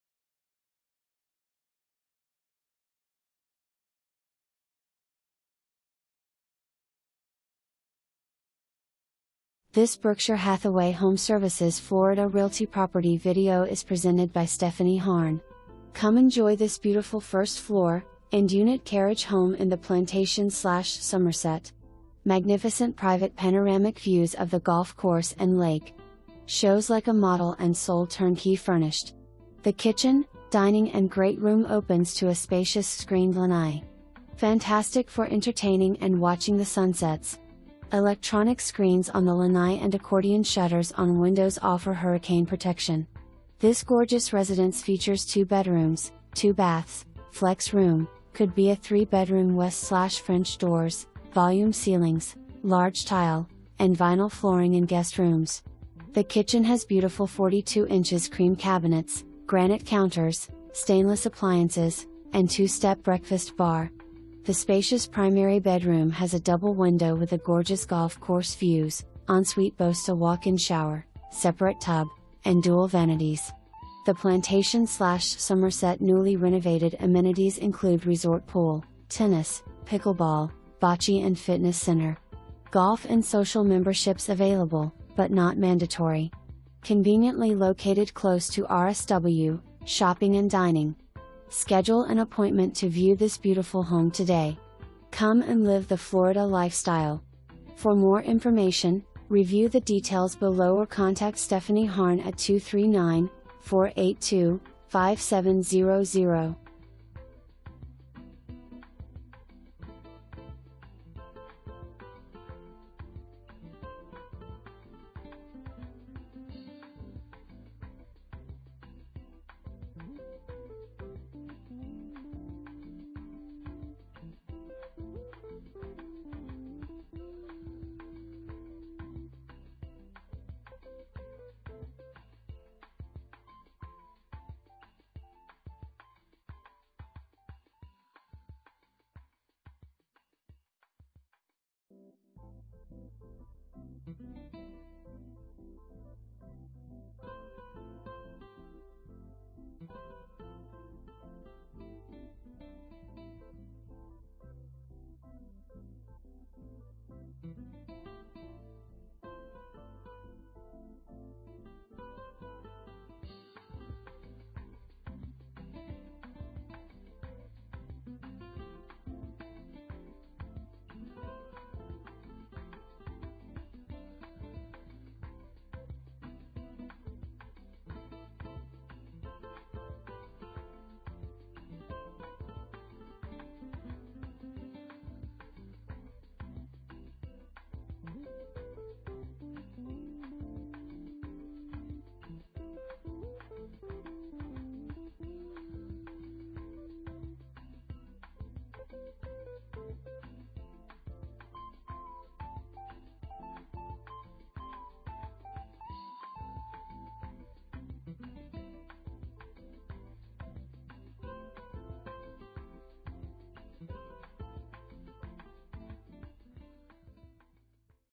Berkshire Hathaway HomeServices Florida Realty - 10311 Glastonbury Circle 101
Come enjoy this BEAUTIFUL FIRST FLOOR, END UNIT CARRIAGE HOME in The Plantation/Somerset. MAGNIFICENT PRIVATE PANORAMIC VIEWS OF THE GOLF COURSE AND LAKE. Shows like a model and SOLD TURNKEY FURNISHED! The kitchen, dining and great room opens to a spacious screened lanai. Fantastic for entertaining and watching the sunsets! Electronic screens on the lanai and accordian shutters on windows offer hurricane protection. This gorgeous residence features 2 Bedrooms, 2 Baths, Flex Room (could be a 3 Bedroom w/French Doors), volume ceilings, large tile, and vinyl flooring in guest rooms. The kitchen has beautiful 42" cream cabinets, granite counters, stainless appliances, and 2 step breakfast bar. The spacious Primary BR has a double window with the gorgeous golf course views, ensuite boasts a walk-in shower, separate tub, and dual vanities. The Plantation/Somerset NEWLY RENOVATED AMENITIES include resort pool, tennis, pickleball, bocce and fitness center. Golf and Social memberships available, but not mandatory. Conveniently located close to RSW, shopping and dining. Come and live the Florida Lifestyle.

Stephanie Harne
Berkshire Hathaway HomeServices Florida Realty